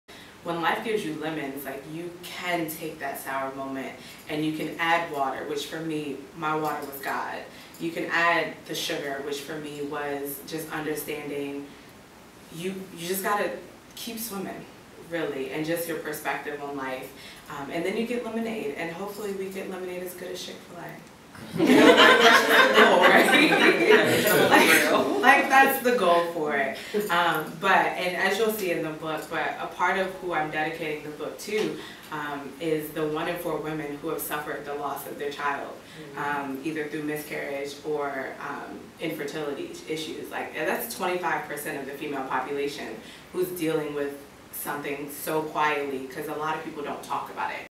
When Life Gives You Lemons... | BookRelease | Tiara Nicole
Here's a clip from the author's 2nd book release "When Life Gives You Lemons..."